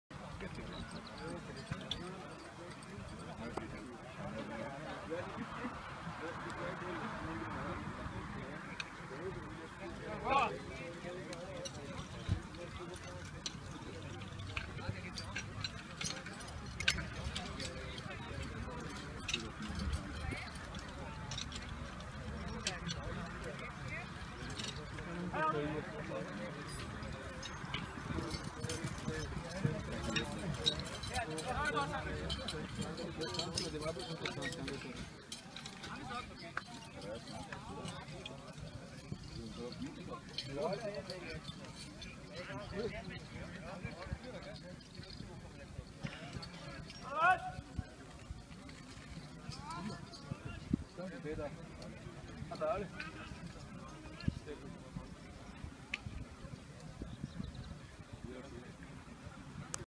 Löschangriff in Bronze 2015 in Wolkersdorf
Löschangriff in Bronze der Wettkampfgruppe FF-Großengersdorf beim Abschnittsfeuerwehrleistungsbewerb in Wolkersdorf am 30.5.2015.